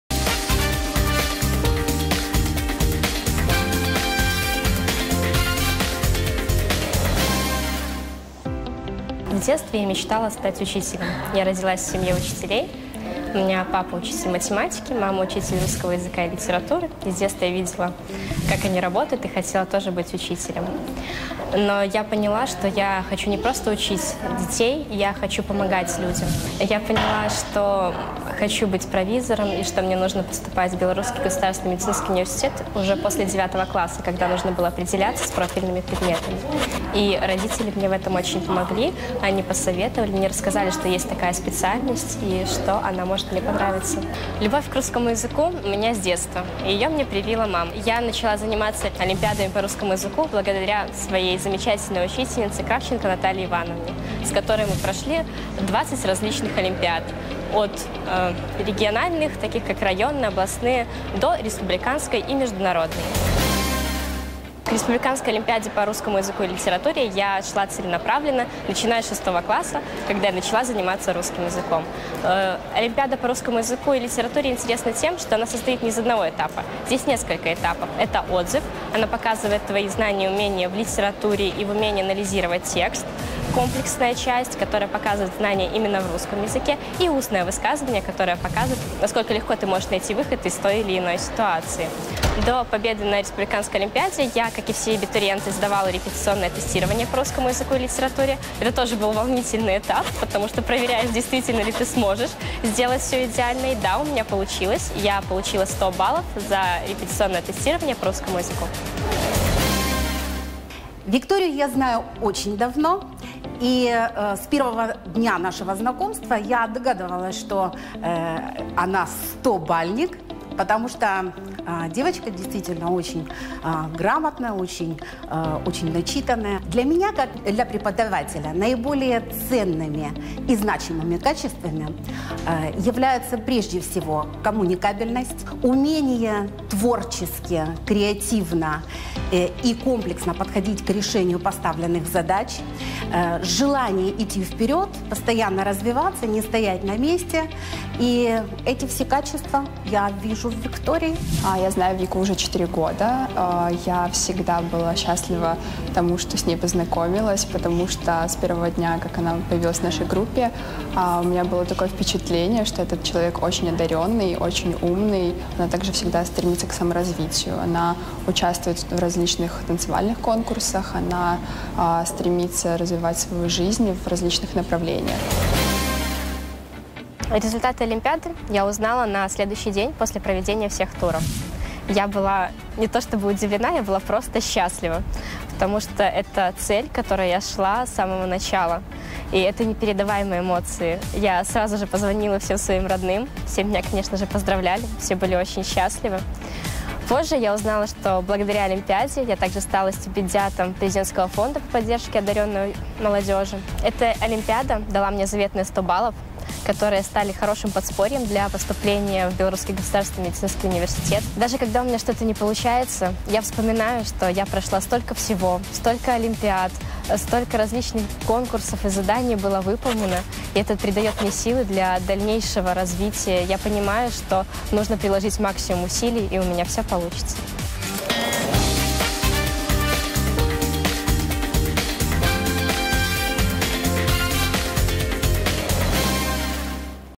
Стобалльники: Виктория Лавшук
В детстве я мечтала стать учителем. Я родилась в семье учителей. Папа учитель математики. Мама учитель русского языка и литературы. С детства я видела, как они работают, и тоже хотела быть учителем.

В 2017 году Виктория автоматически получила 100 баллов на ЦТ по русскому языку.